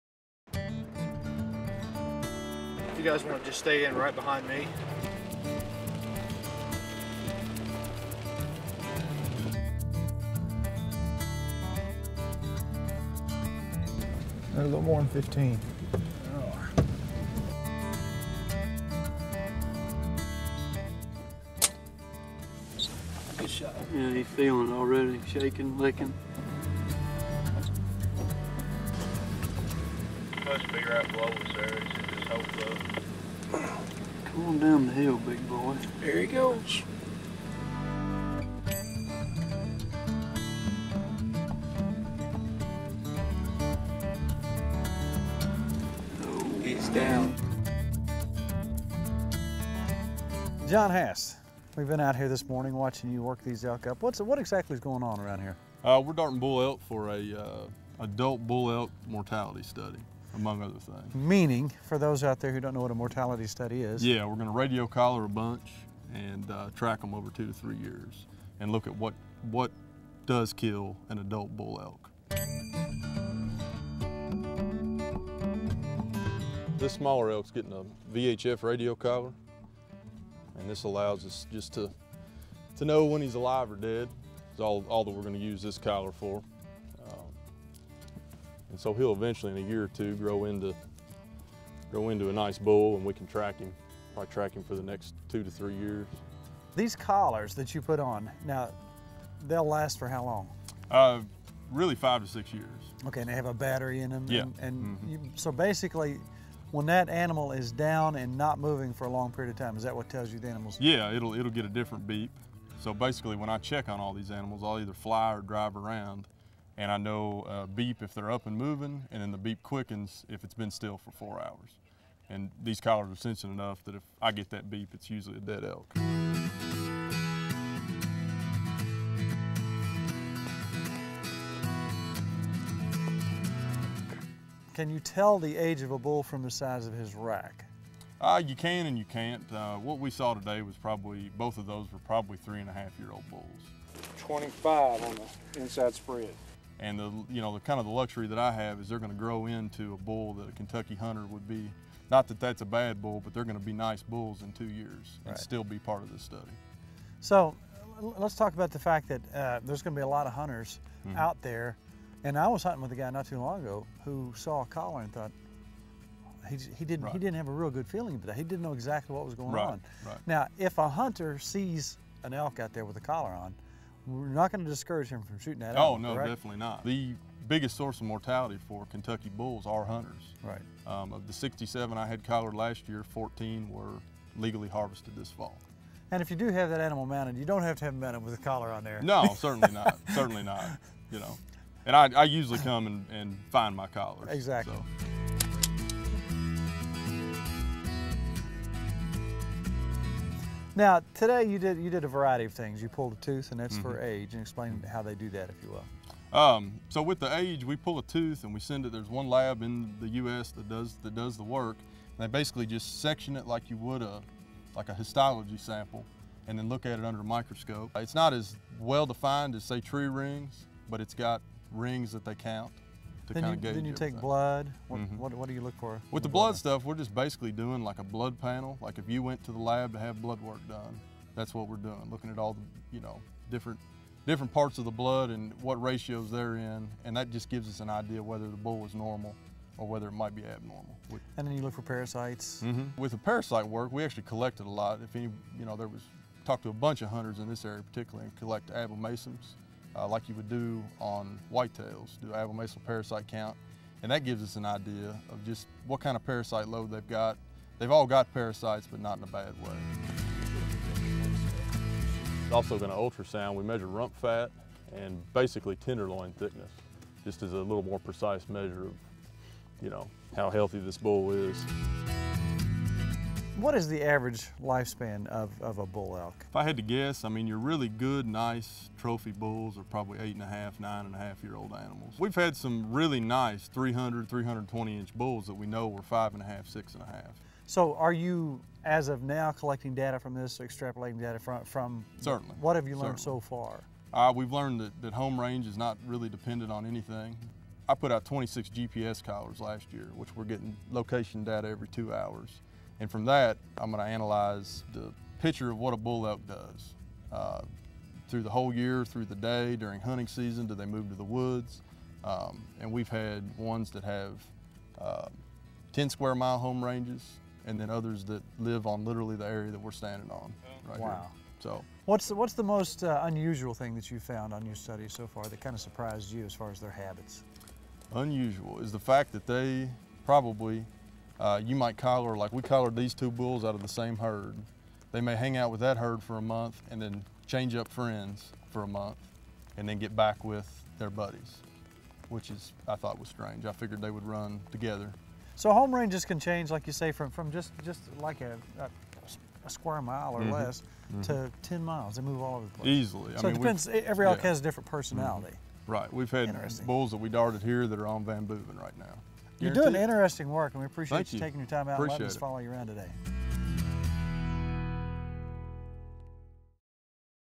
Elk Tagging and Work up
The Kentucky Department of Fish and Wildlife Resources, in order to maintain a healthy population of elk, dart elk and perform a complete work up of information for tracking the health of Kentucky elk.